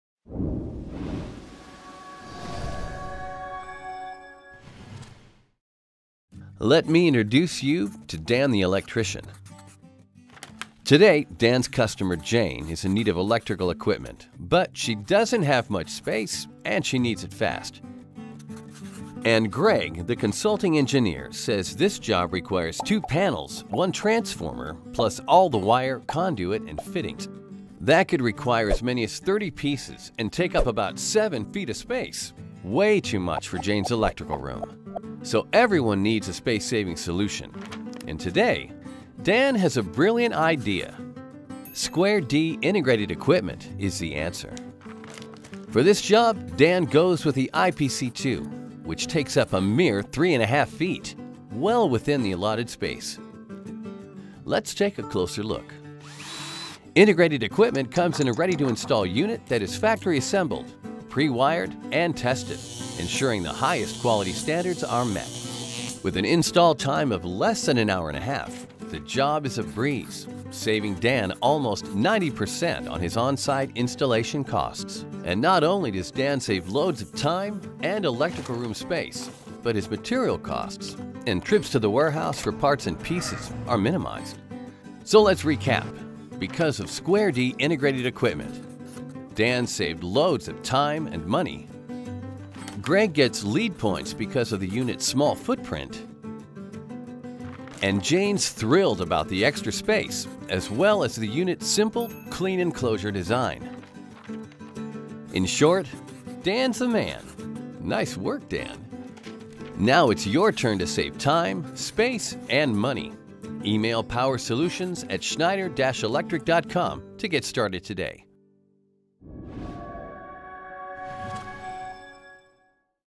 See How Dan the Man Saves Time with Square D™ Integrated Equipment
Learn how to reduce installation time by 90% with Square D™ Integrated Equipment. Pre-wired and factory assembled in one enclosure, installation takes hours instead of days. Modular and scalable design with flexible configurations. Saves up to 40% in wall space. Save time and space!